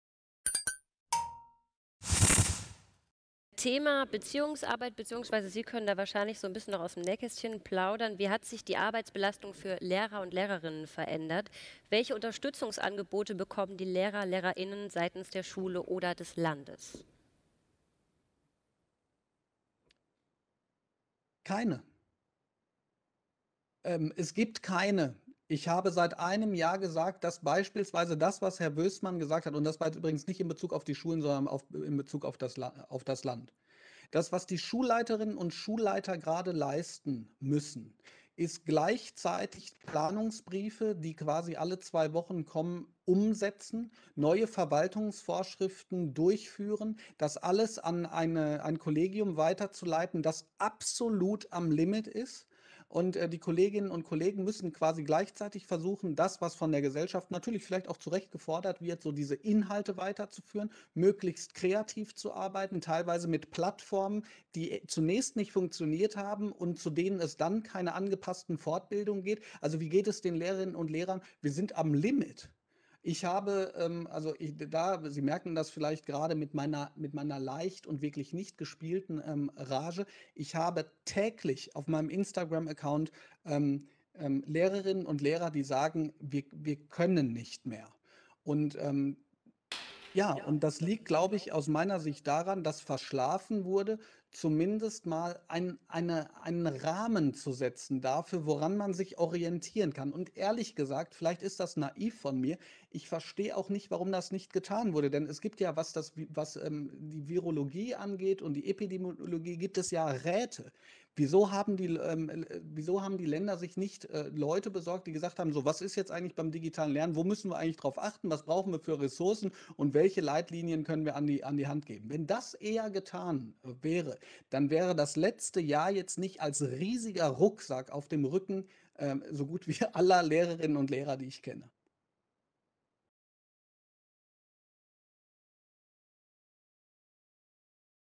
Zitat B. Blume - Science On-Talk: Lernen im Lockdown - Wohin steuert die Bildung?
Statement des Lehrers Bob Blume, Referent für digitale Medien, Offenburg